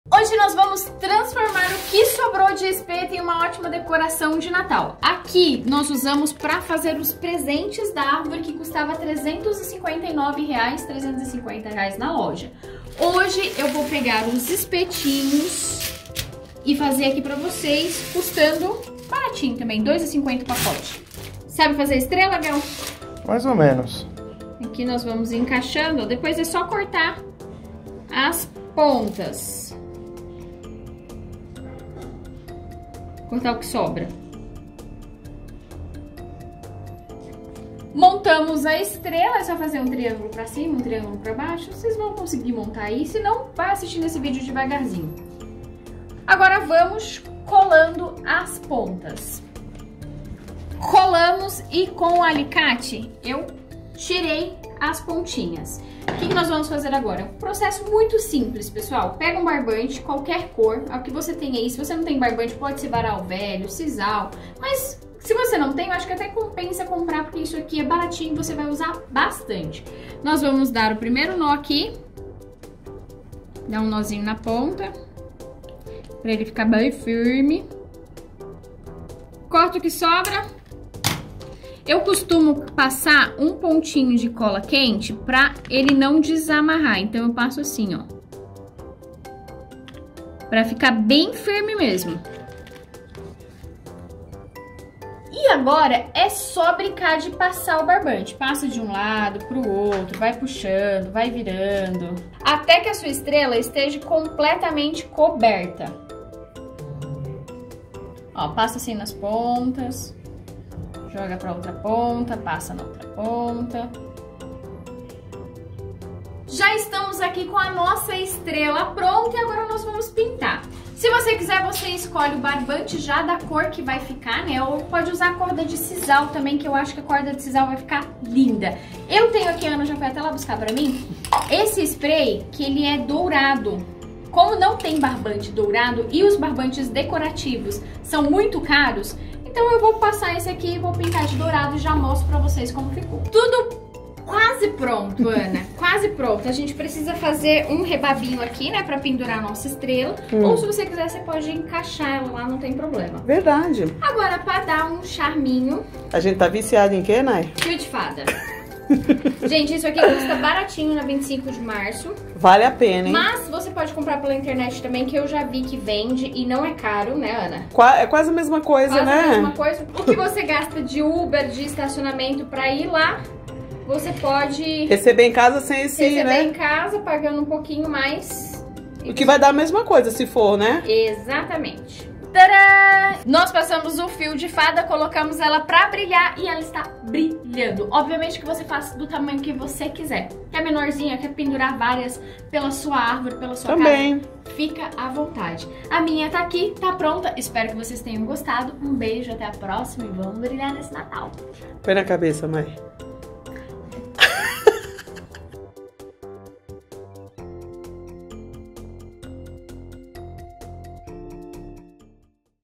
Vamos Transformar sobras de Espetos em uma linda Decoração de Natal ❤️
Com muito pouco você consegue fazer essa linda Estrela de Natal para enfeitar a sua casa ou mesmo a sua Árvore de Natal.